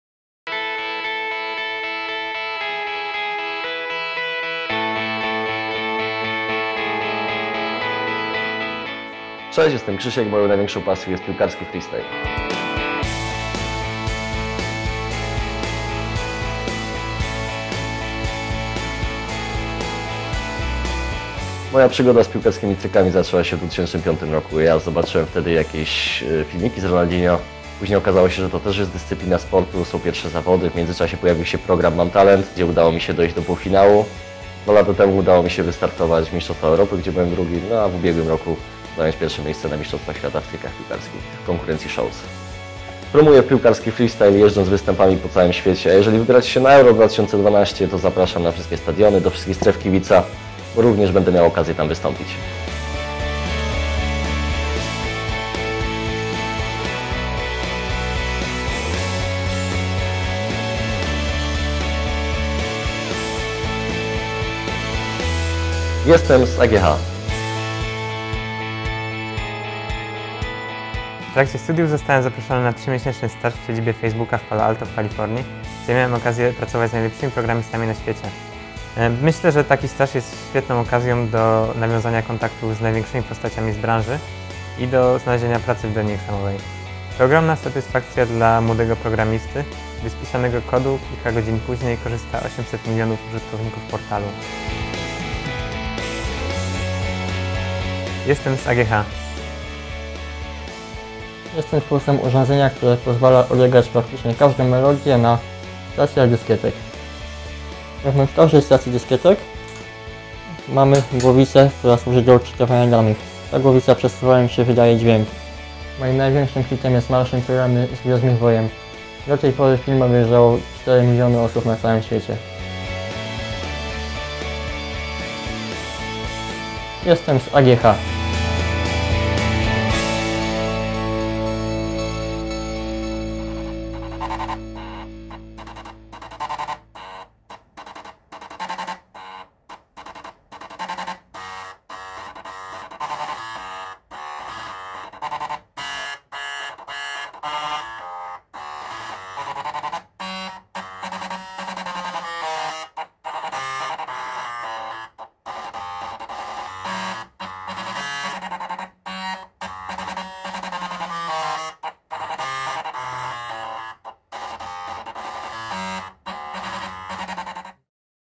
[ TV AGH ] Jestem z AGH - odc. III
Projekt "Jestem z AGH" ukazuje studentów i absolwentów naszej uczelni oraz ich interesujące, często niezwykłe pasje. W trzecim odcinku mistrz świata w trikach piłkarskich, stażysta Facebooka oraz twórca hitów serwisu YouTube.

Realizacja: Wojciech Wojciechowski & URSSTV AGH
Idea: BD&AŻ